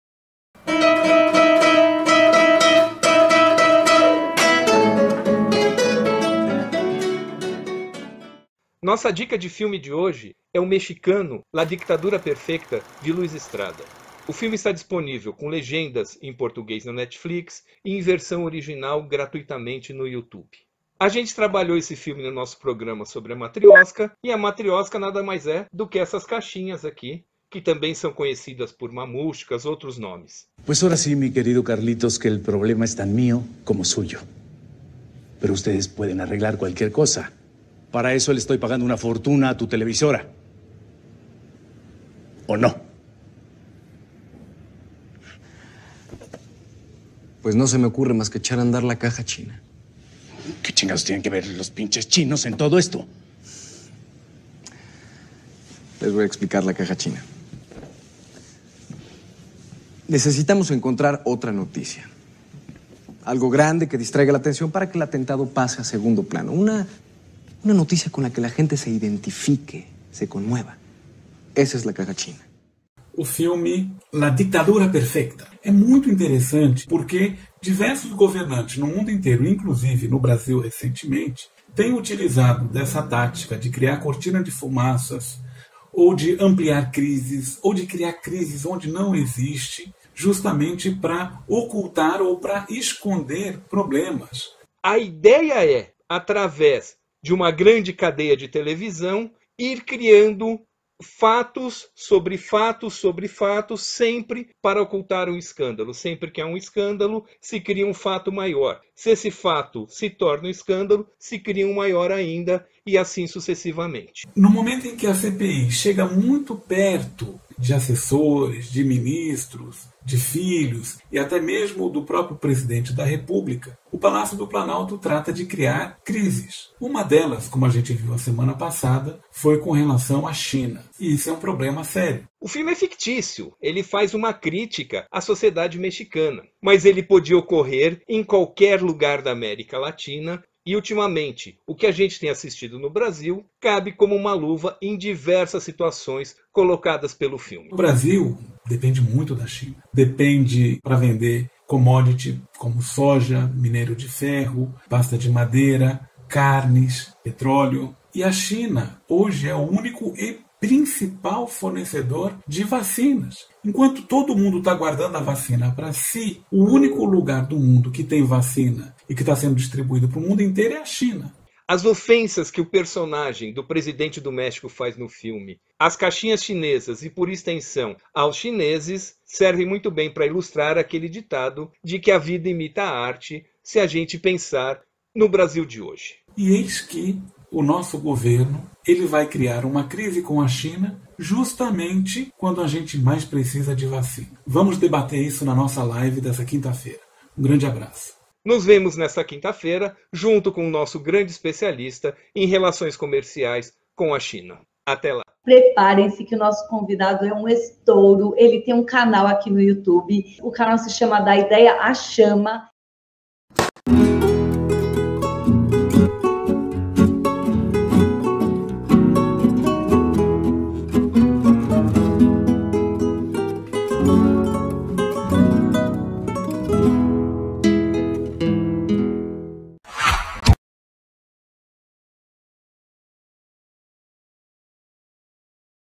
China e vacina: e eu com isso? ISKRA #33
Chamada para a live a ser realizada 5ªf 13/05 às 21hs, debatendo a crise dos insumos chineses para a fabricação de vacinas no Brasil, a partir do conceito de Matrioska da Informação (ou caixas chinesas), proposto pelo filme "La dictadura perfecta" de Luis Estrada.

   obs: disponível também na Netflix com legendas